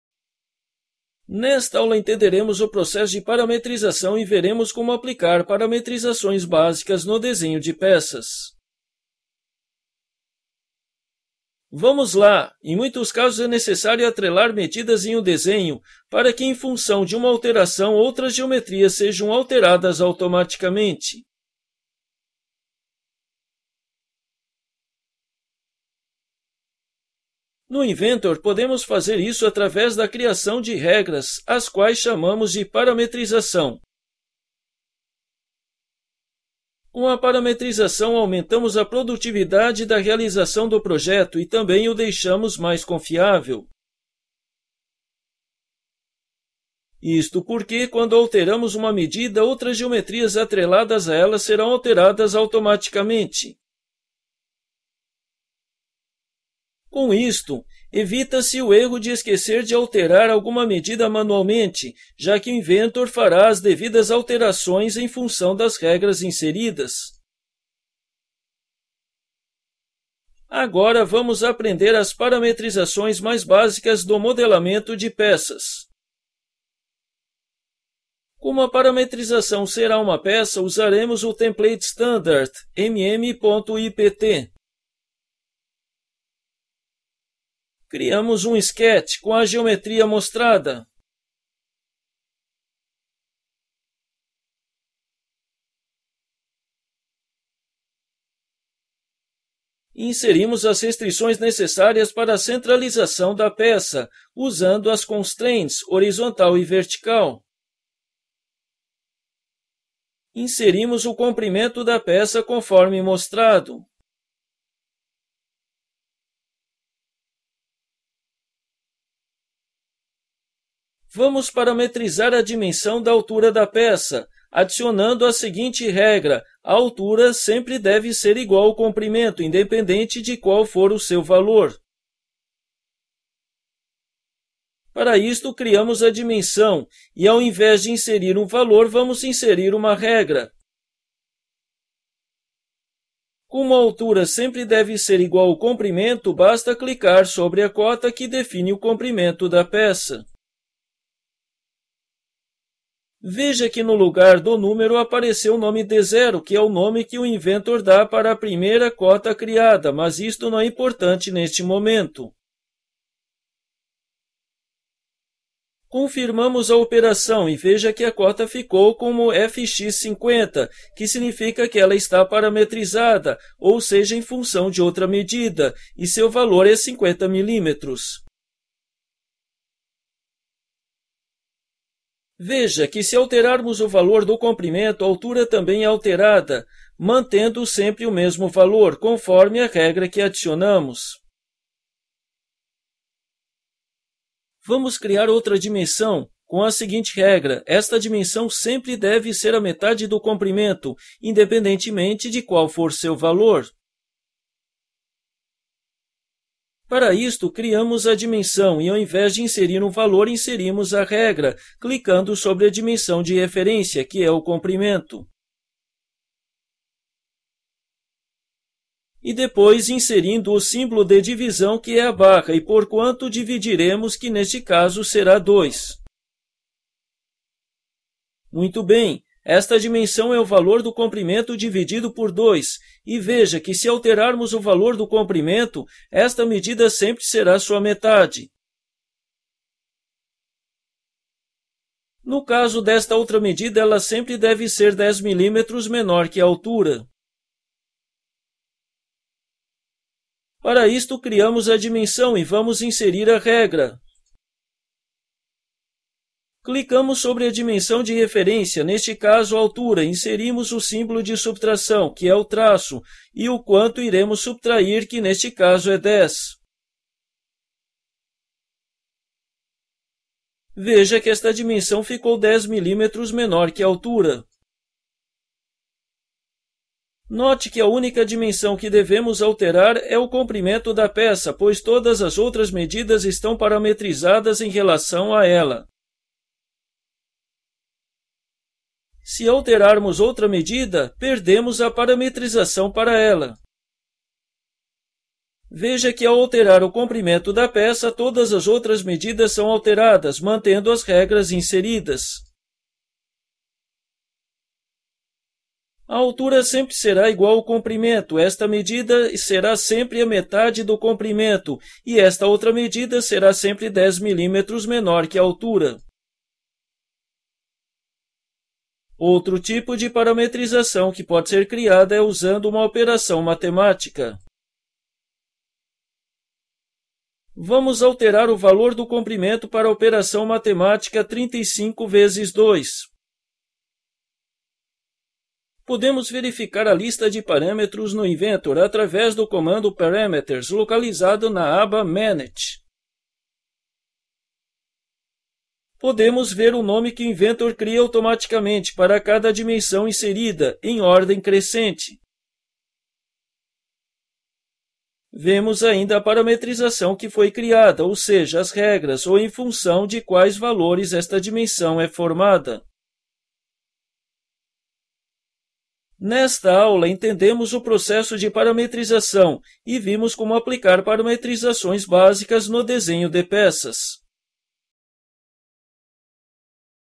Curso Inventor 2017 Projetos Paramétricos - Entendendo o Conceito da Parametrização
Com videoaulas práticas e dinâmicas, este curso ensina técnicas para otimizar projetos no Inventor e aumentar a produtividade, através da parametrização. Saiba parametrizar e relacionar dimensões em peças, restrições de montagem e dimensões de peças dentro de montagens. Crie parâmetros no Excel e importe no Inventor, adicione regras e padronizações de montagens com iMates e muito mais. Faça os exemplos e exercícios propostos e esteja capacitado para atuar em projetos altamente requisitados!

Nesta aula entenderemos o processo de parametrização e veremos como aplicar parametrizações básicas no desenho de peças.